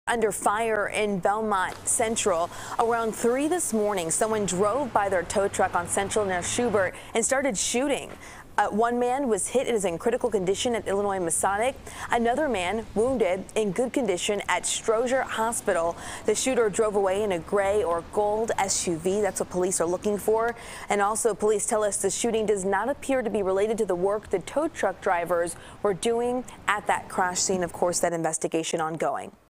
2 Tow Truck Drivers Shot At Crash Scene in Belmont Central
Two tow truck drivers were shot while on a crash scene in the Belmont Central early Thursday morning.